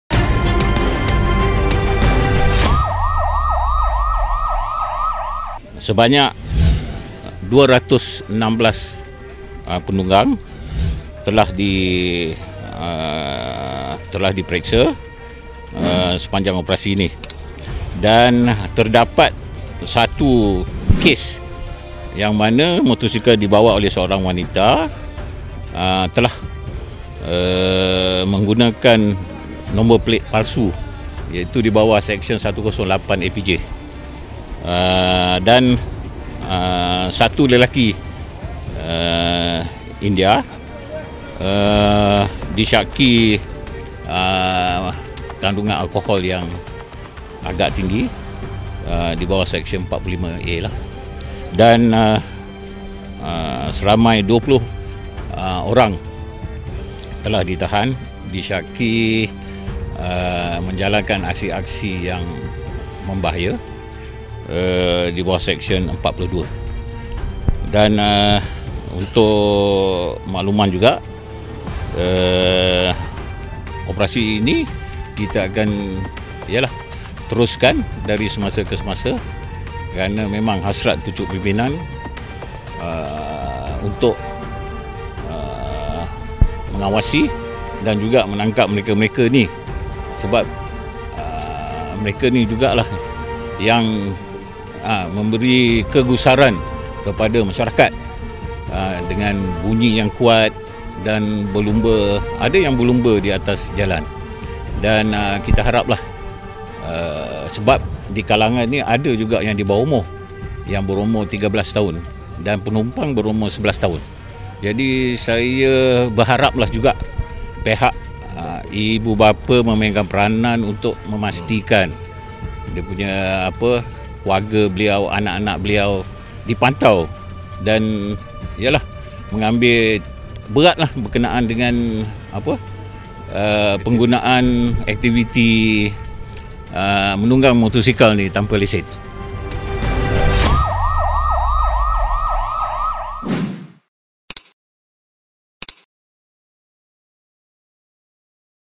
MKL Crimedesk | Samseng Jalanan Tak Hirau PKPP
Sudah lah tak hiraukan perlaksaan Perintah Kawalan Pergerakan Pemulihan.

Malah kebal pula di jalanan buat aksi merbahaya ketika menunggang motosikal.

Itu kegiatan sekumpulan pemuda yang akhirnya ditangkap dalam OPS Samseng Jalanan dekat Plaza Tol Damansara Puchong malam tadi.

Operasi dilaksanakan Jabatan Siasatan Penguatkuasaan Trafik Bukit Aman itu mencekup 20 pemuda yang menunggang secara merbahaya termasuk remaja seawal usia 11 hingga 13 tahun.

Dengan kekuatan 12 pegawai dan 65 anggota termasuk Agensi Anti Dadah Kebangsaan dan Konsesi Litrak... Laluan di kedua dua arah di kepong bermula jam 12.30 pagi.

Sekurang kurangnya 216 penunggang motosikal ditahan untuk pemeriksaan.

Ketika ditahan.. Ada yang cuba melawan arus.

Operasi membabitkan 16 buah pelepasan Kereta Peronda (MPV) dan 3 Motosikal Unit Rondaan Bermotosikal (URB) dan 3 kenderaan Konsesi itu turut mencekup 20 samseng jalan yang menunggang dengan aksi berbahaya.

Hasil semakan turut menemui 1 kes nombor plat palsu yang ditunggang oleh seorang wanita.

Wanita itu ditangkap dan siasatan dilakukan mengikut Seksyen 108 Akta Pengangkutan Jalan 1987.

Seorang lelaki tempatan juga ditahan selepas didapati menunggang dalam pengaruh alkohol melibihi had yang ditetapkan.

Lelaki itu disiasat mengikut Seksyen 45A (1) Akta Pengangkutan Jalan 1987.

20 pemuda yang melakukan aksi berbahaya itu ditahan di bawah Seksyen 42 Akta Pengangkutan Jalan 1987.

Operasi berakhir jam 3 pagi.